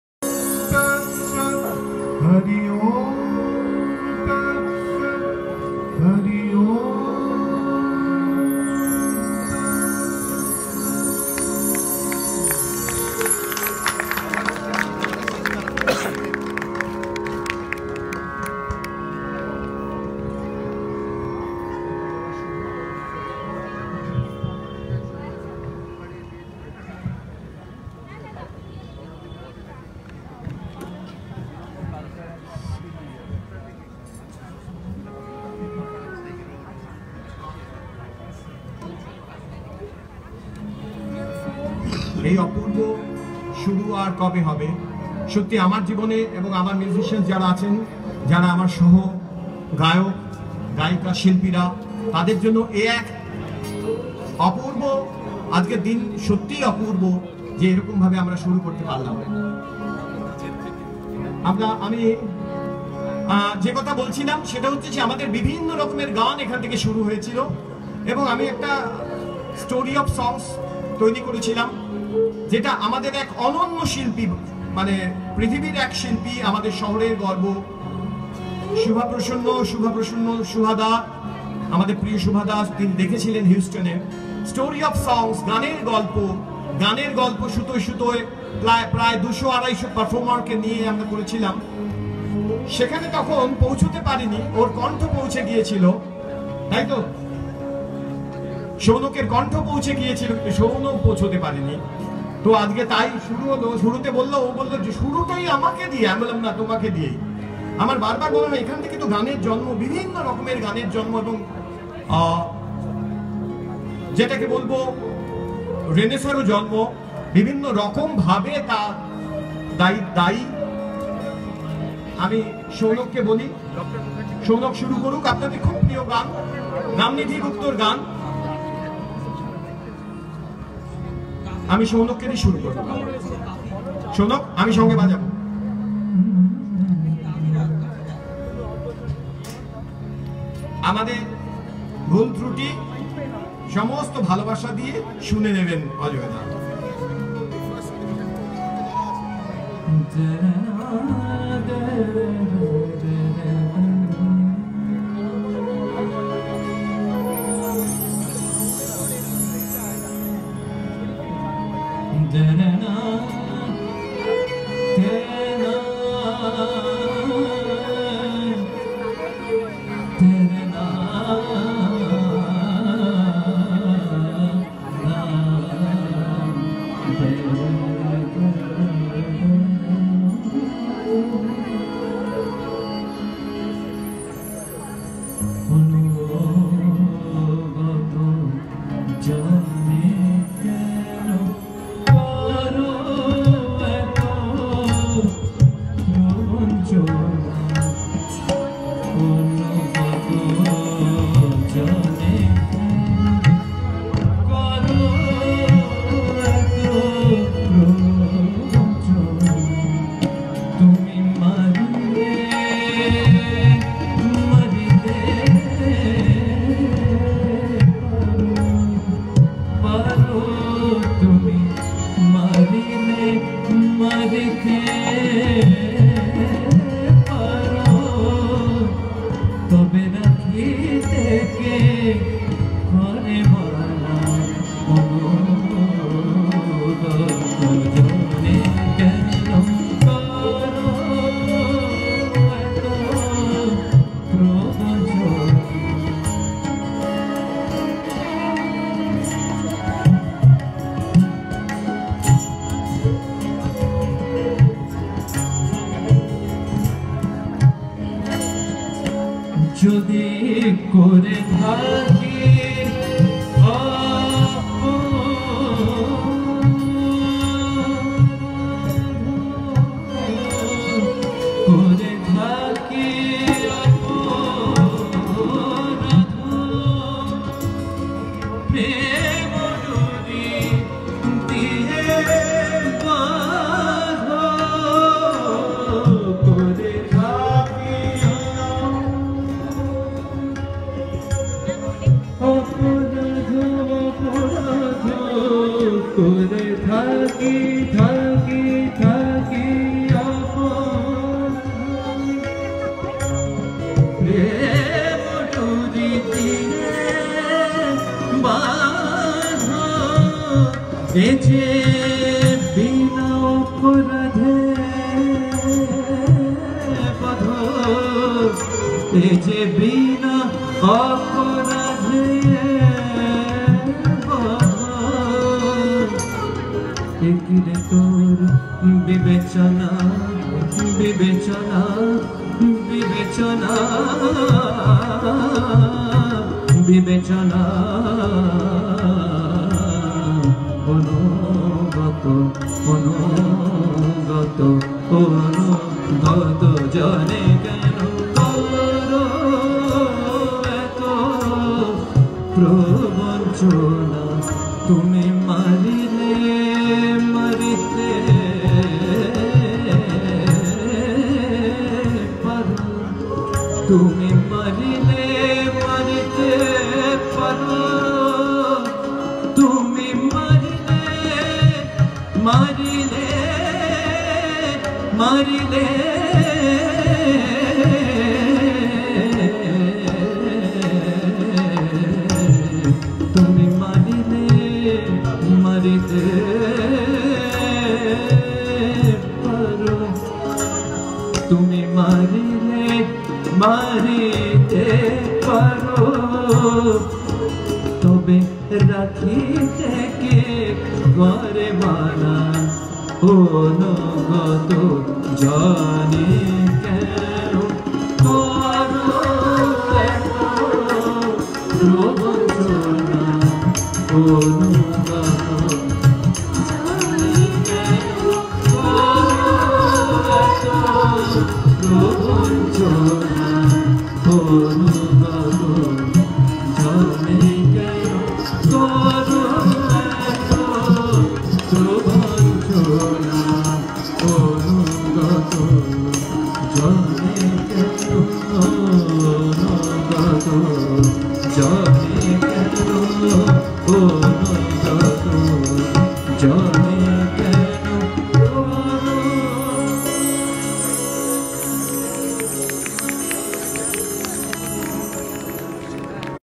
Ajoy Chakrabarty | Debojyoti Mishra | Rosogolla Utsab | Jiyo Bangla
Hear the mesmerizing voice of Ajoy Chakrabarty and Debojyoti Mishra as they perform during the Rosogolla Utsab at Bagh Bazaar.